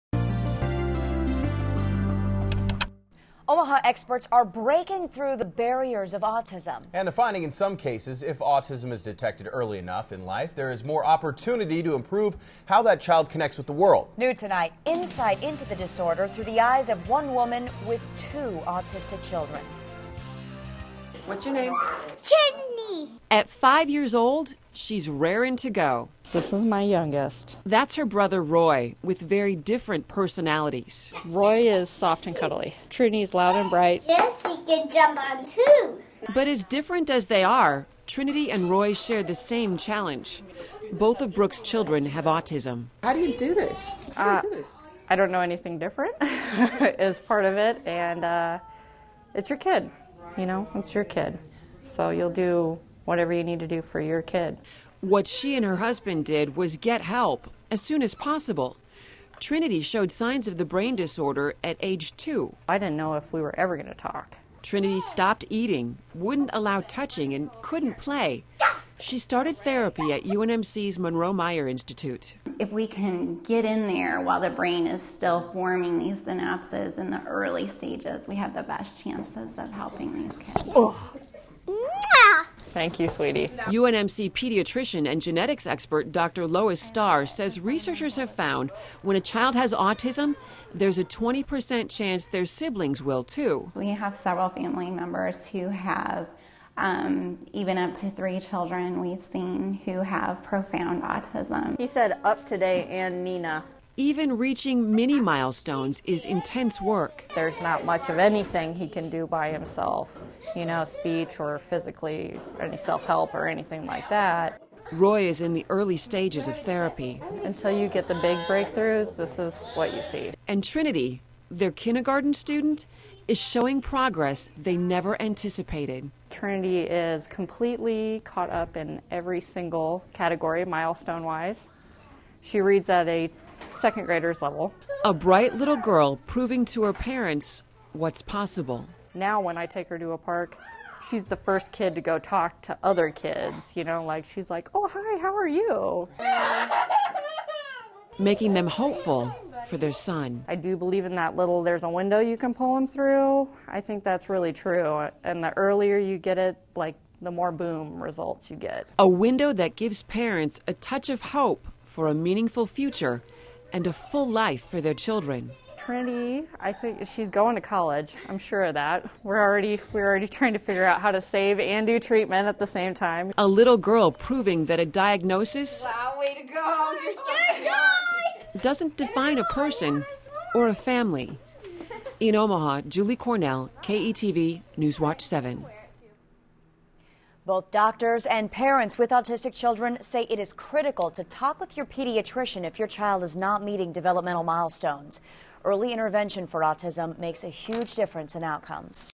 Omaha Doctors Breaking Barriers Of Autism
Omaha experts are finding, in some cases, if autism is detected early enough in life, there are more opportunities to improve how a child connects with the world.